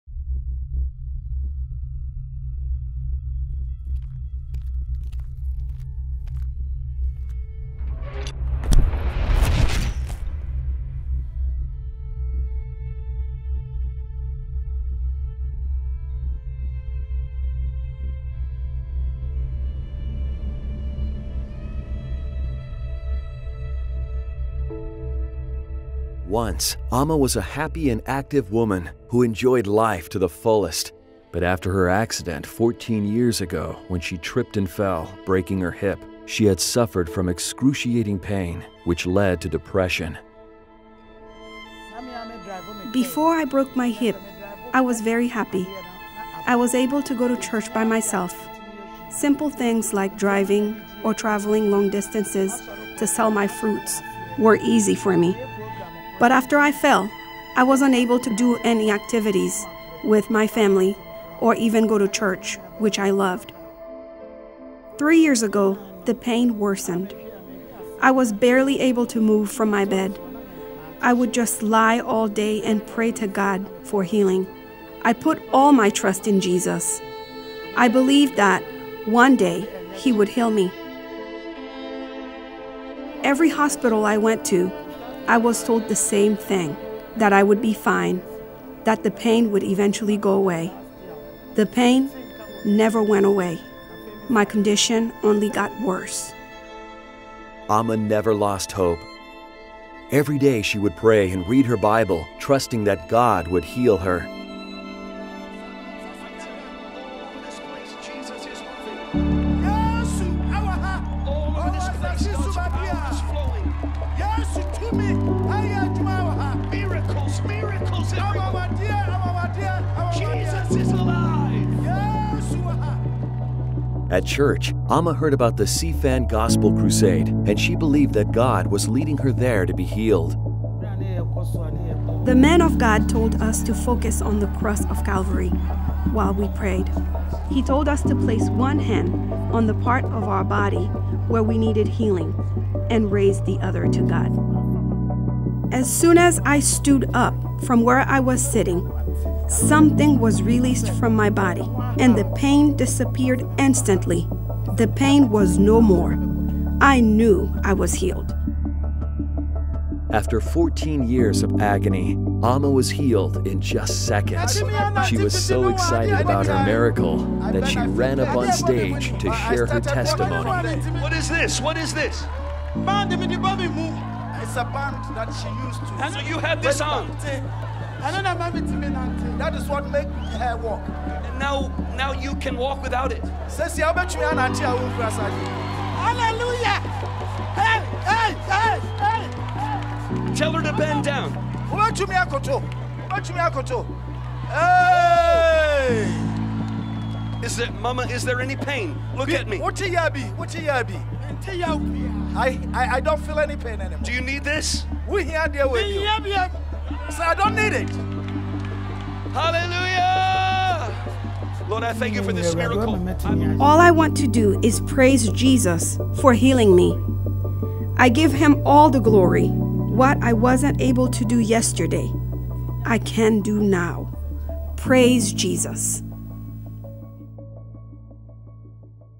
She Removed Her Back Brace and Danced!
After Amma Takyiyan broke her hip in an accident she suffered for 14 years with excruciating pain. She spent most of her days lying down and when she walked, it was with great difficulty and not without her back brace. When she attended the Christ for all Nations gospel campaign in Kumasi, Ghana, she received an amazing miracle.